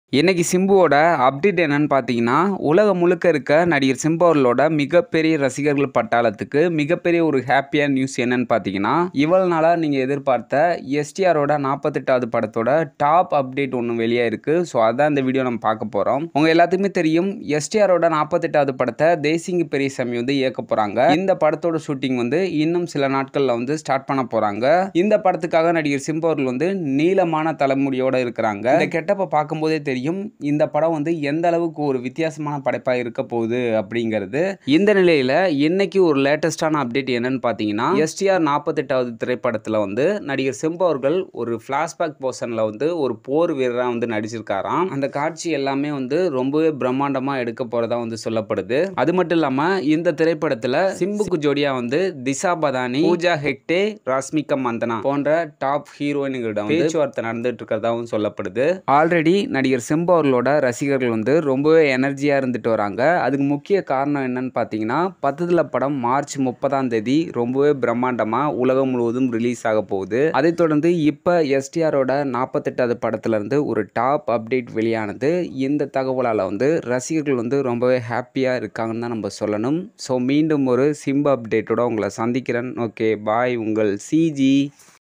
VTK சிம்பு 48 படத்துல போர் வீரர் சிம்பு | ஜோடியாக நடிக்க 3 ஹீரோயின்கள் | ரசிகர்கள் பட்டாளம்
சிம்பு ரசிகர்களுக்கு வணக்கம்

என்னுடைய பெயர் பார்த்திபன். சி

என்னுடைய மாவட்டம் கரூர்

SimbuFans, HeroCgInfo